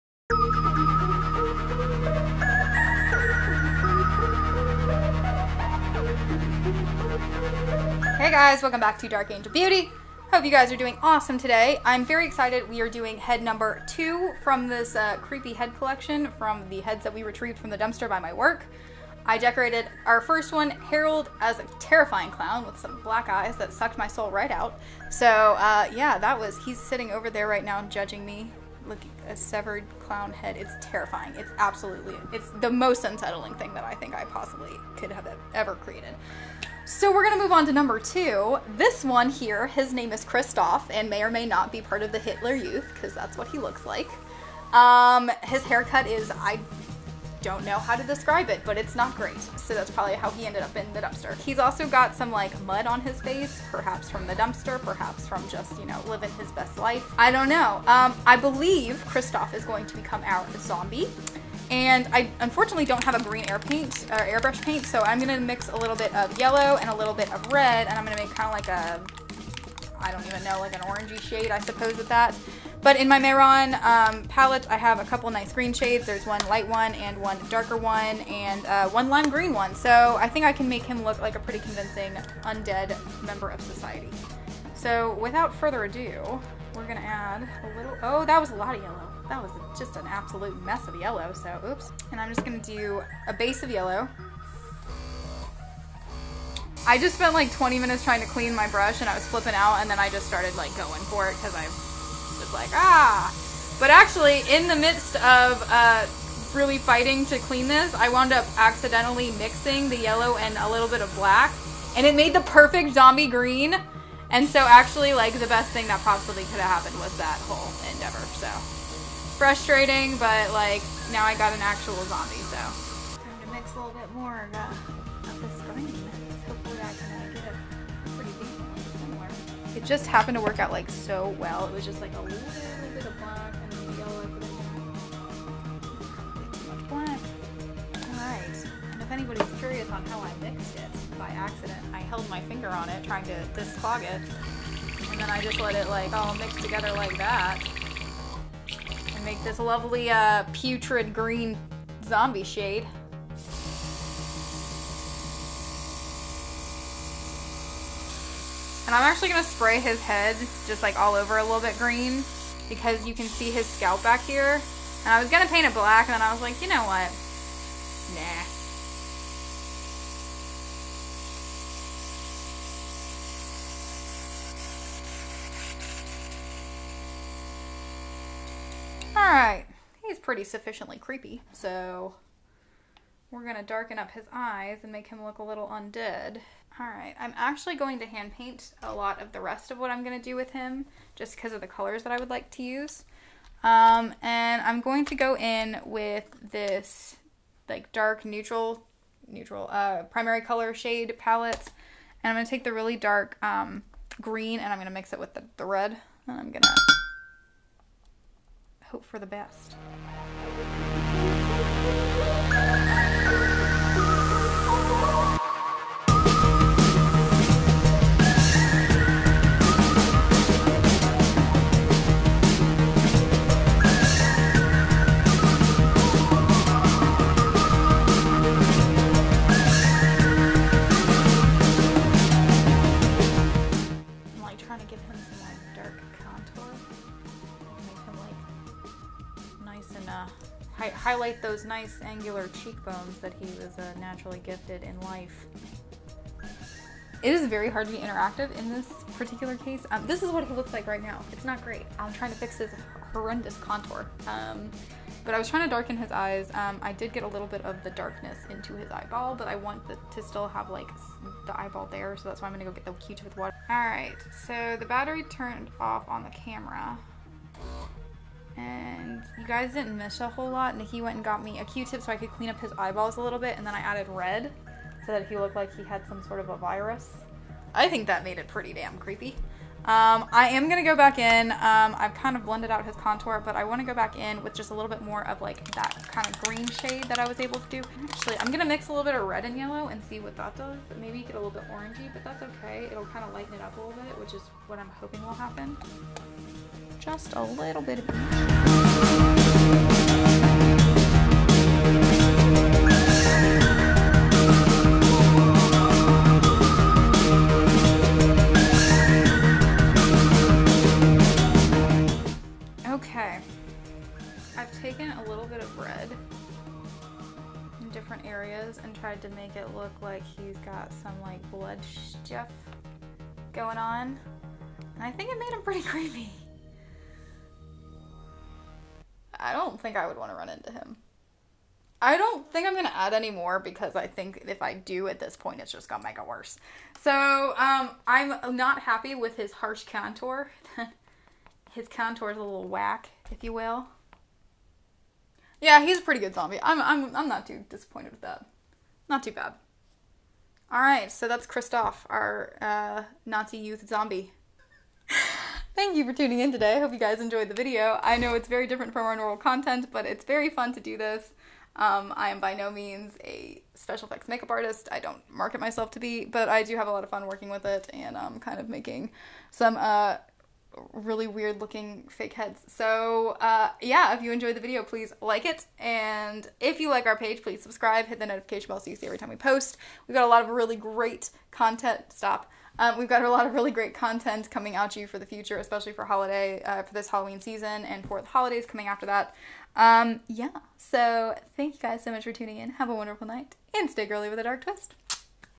KRISTOPH THE ZOMBIE | AIRBRUSH PAINTING
Here's the second head I painted for Halloween! Meet Kristoph the Zombie!

As always; stay girly, but with a dark twist!

Equipment:

Camera: Nikon D3300
Lens: Nikon 18mm - 55mm zoom lens
Audio: Zoom H6 Portable Recorder
Ring Light: Neewer Ring Light Kit